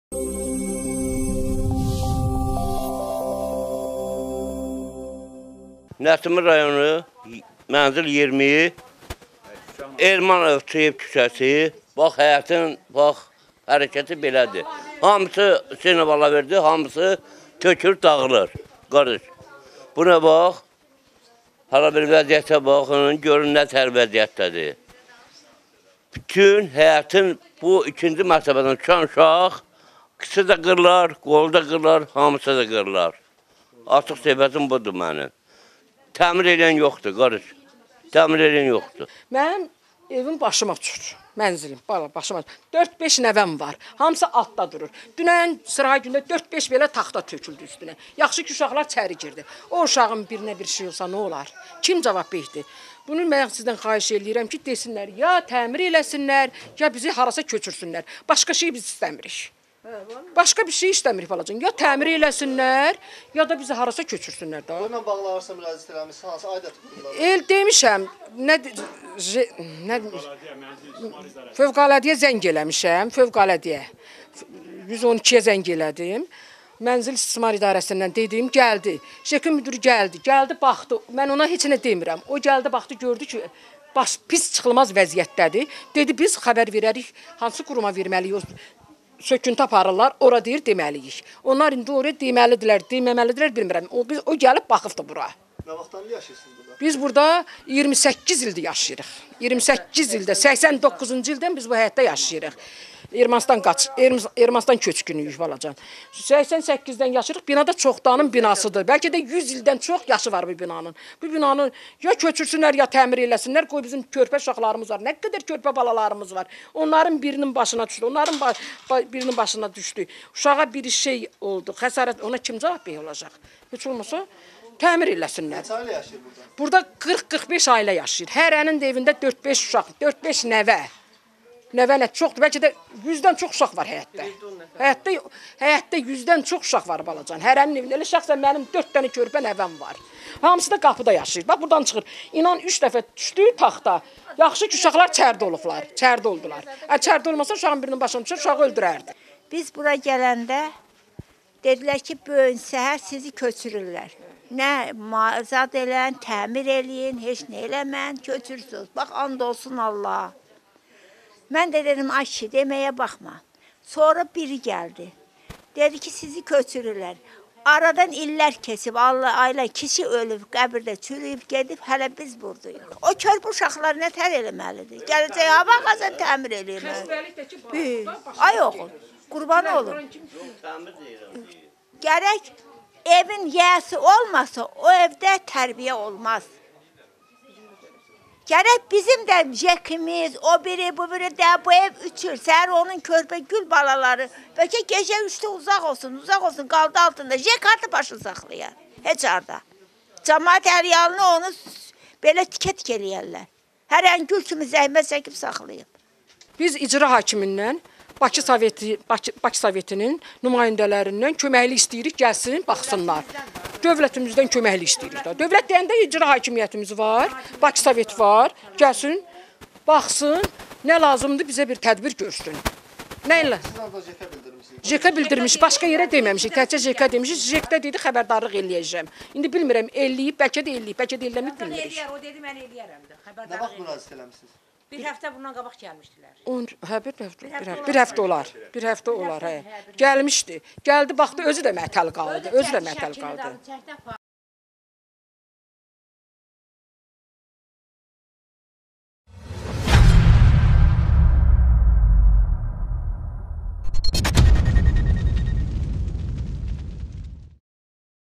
"Kişi ölüb, çürüyüb gedib, biz hələ burdayıq" - şikayətçi Samara Fərəcova
Kanal13-ə müraciət edən Nəsimi rayonu Elman ovçuyev küçəsinin sakinləri
evlərinin qəzalı vəziyyətdə olmasından şikayətlənirlər.
Ümumilikdə 45-ə yaxın ailənin yaşadığı məhəllənin təqribi 100 dən çox yaşı var.
Ermənistan Respublikasından olan qaçqınlar və məcburi köçkünlər bu məhəllədə 1989-cu ildən bəri yaşayırlar.
Onlar mövcud durumla bağlı Fövqəladə Hallar Nazirliyinin 112 nömrəli qaynar xəttinə və Mənzil - Kommunal İsrismar Sahəsinə müraciət
etsələr də hələ ki, bir müsbət cavab almağa nail olmayıblar.